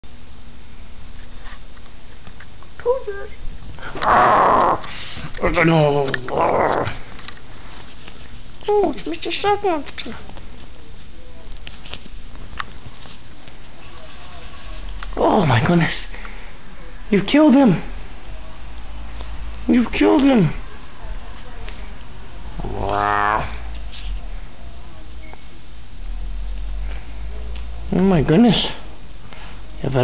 Sock Monster vs. Toonces
Toonces battles the sock monster and kills him.